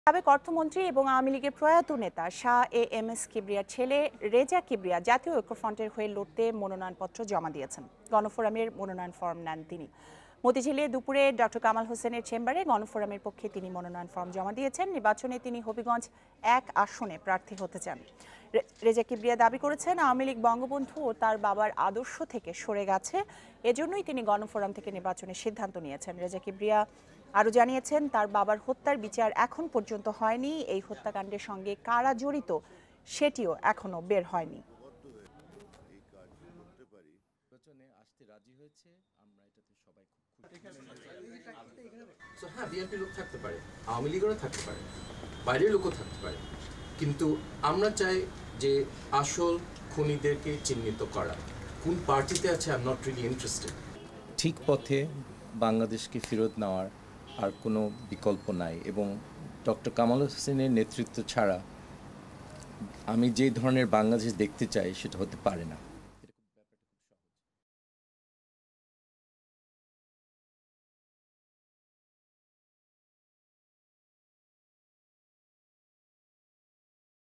ড. কামালের হাতে রেজা কিবরিয়ার মনোনয়ন ফরম II Reza Kibria joins Jatiya Oikyafront
সাবেক অর্থমন্ত্রী ও আওয়ামী লীগের প্রয়াত নেতা শাহ এ এম এস কিবরিয়ার ছেলে রেজা কিবরিয়া জাতীয় ঐক্যফ্রন্টের হয়ে লড়তে মনোনয়নপত্র জমা দিয়েছেন। গণফোরামের মনোনয়ন ফরম নেন তিনি।